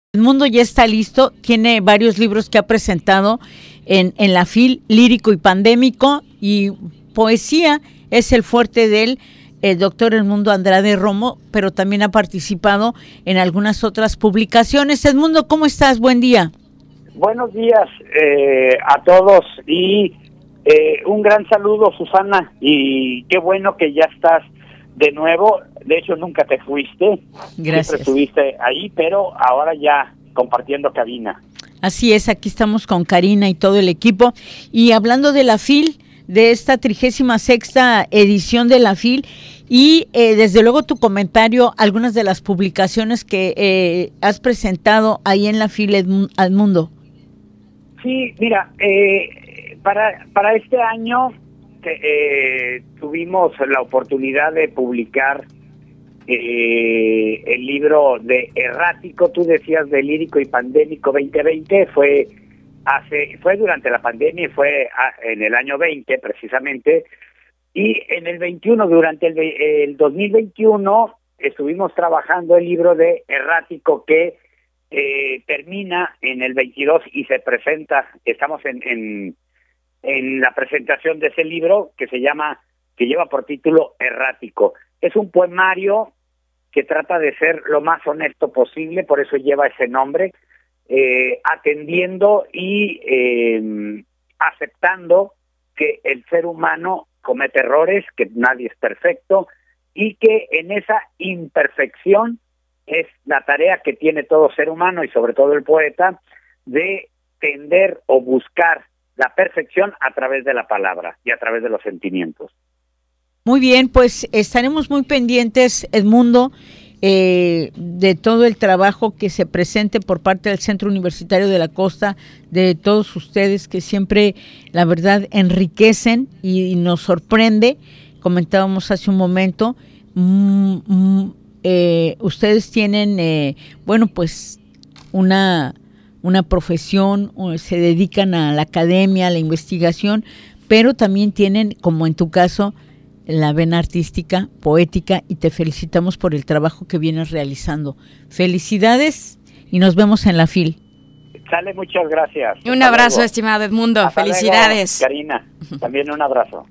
ENTREVISTA CON: Dr. Edmundo Andrade Romo, presentación de su libro Errático en la FIL
ENTREVISTA CON: Dr. Edmundo Andrade Romo, escritor, académico e investigador del Centro Universitario de la Costa para hablar de la presentación de su libro Errático en la Feria Internacional del Libro